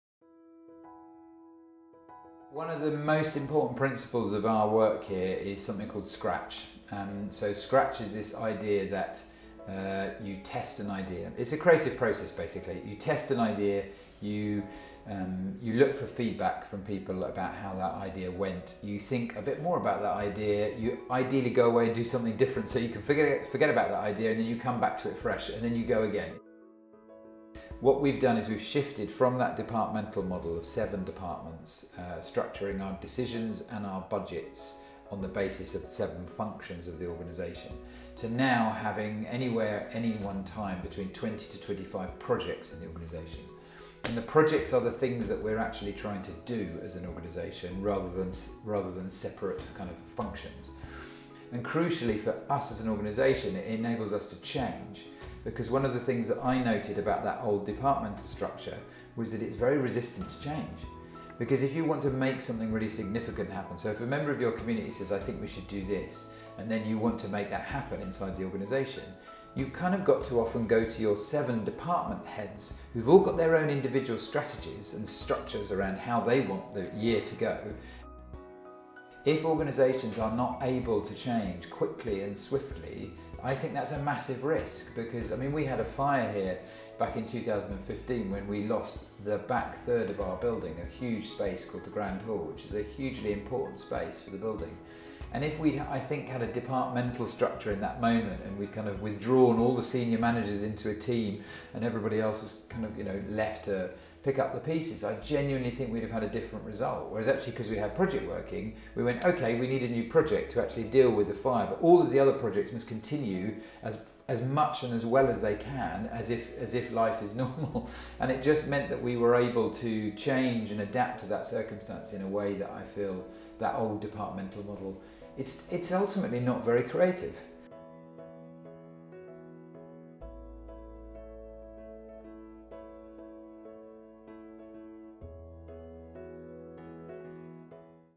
Creative process is at the heart of everything we do
Battersea Arts Centre
Interviewee: David Jubb

This video is part of the Unusual Suspects digital storytelling project, developed in collaboration with Spread-i, C. and Seoul Innovation Park. The six chapters of unusual suspects tell the stories of individuals, and the change they’re making in their surroundings.